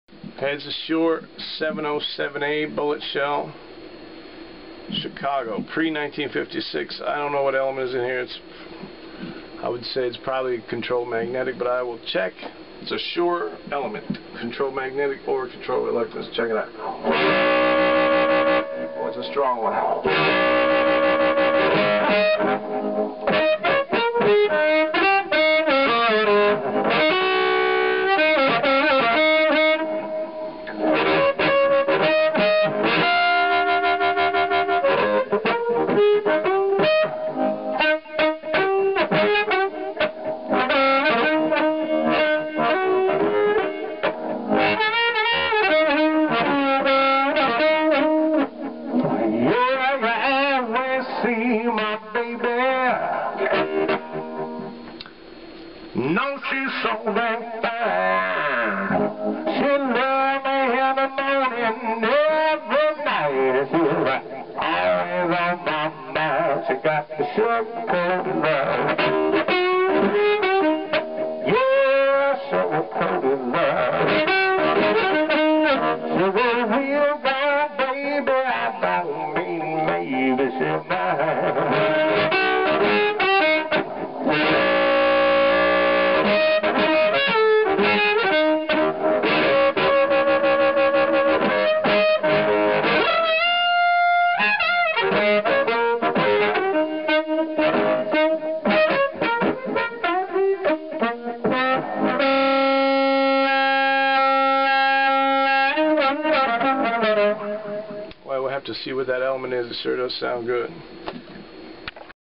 Custom Shure 707A Bullet Mic with 1960 Controlled Magnetic Element for Harp and Vocals
(Bookmark this link):

This is a warhorse 707A shell (no vent holes for maximum airtightness and tone) that has been customized with a 1960 High Impedance Shure 99G86 Controlled Magnetic Transducer, Switchcraft 5/8" screw-on connector, new Harp Mic Gasket, and all new wiring.  Stylish purple grill silk.  The original paint shows its age (pre-1956) and so does the grill.  The tone is magnificent, and this custom job by master builder Jeremy Snell will give its new owner years of pleasure.  Great for harp, vocals, or any other use you can think of.

I sometimes also use a vintage Silvertone 1432

This mic will be listed on Ebay in the second week of September, 2010.